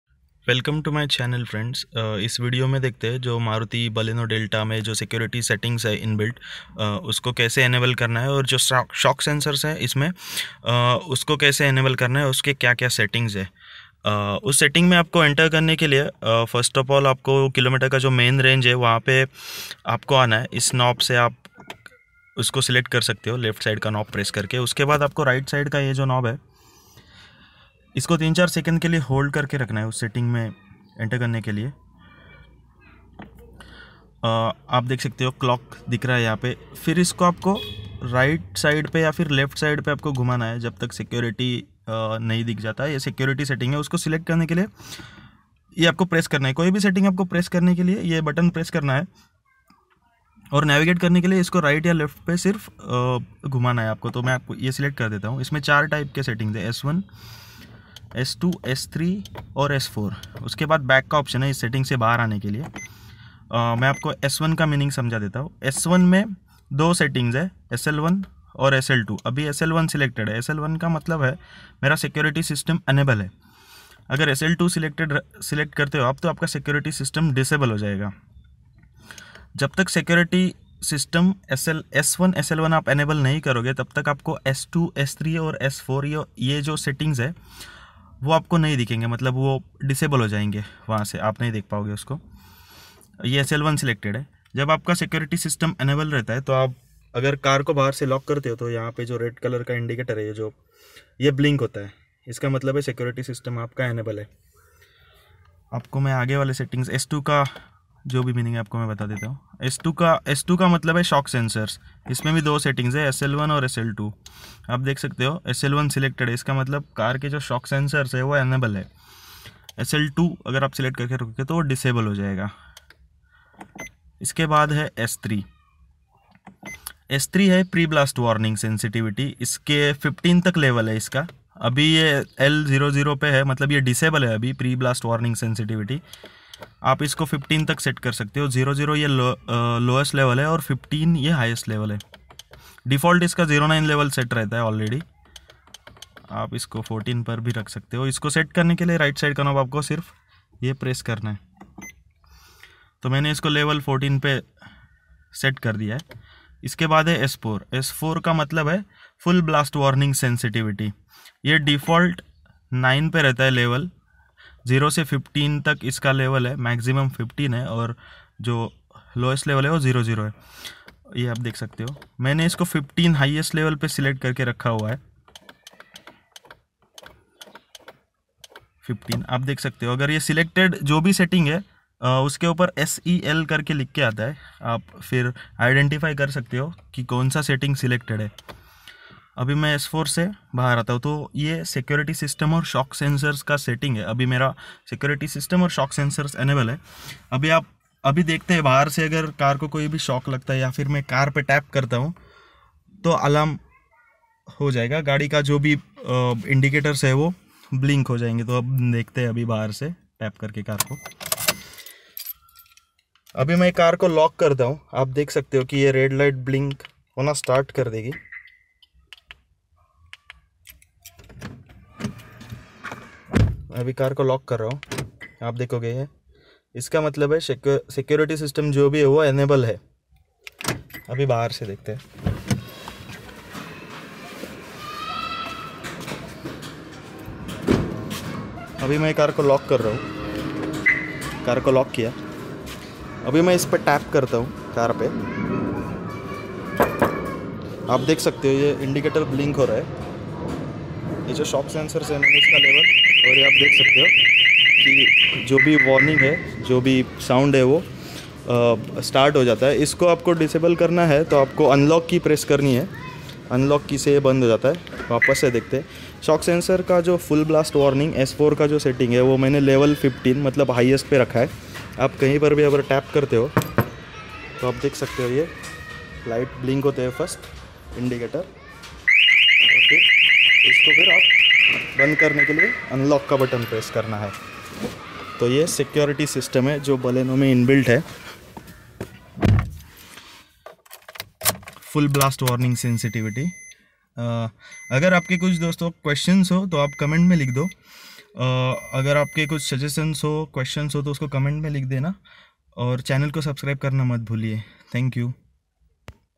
Baleno Delta shock sensors and Security system
You can activate security and shock sensors which is inbuilt feature in Baleno Delta.